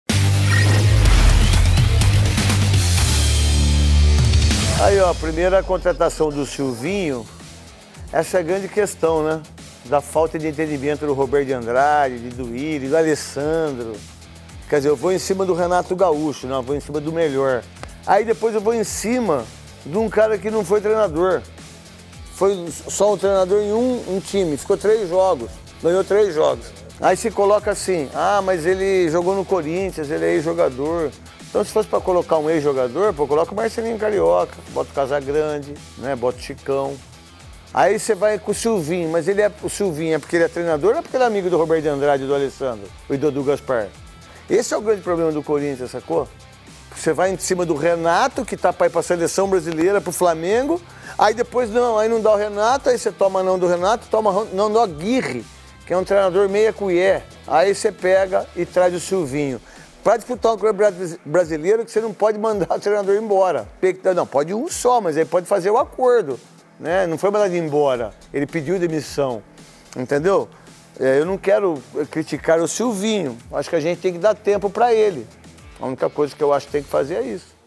CRAQUE NETO NÃO APROVA SYLVINHO NO CORINTHIANS | PLANTÃO DO CRAQUE NETO #4
Fala, garotinho! A contratação do Sylvinho só mostra o quanto a diretoria do Corinthians é perdida. Eles tentam o Renato, que é o cara, e depois fecham com um cara que nem é treinador direito.

Se liga na programação:
Às terças rola o “Neto Responde”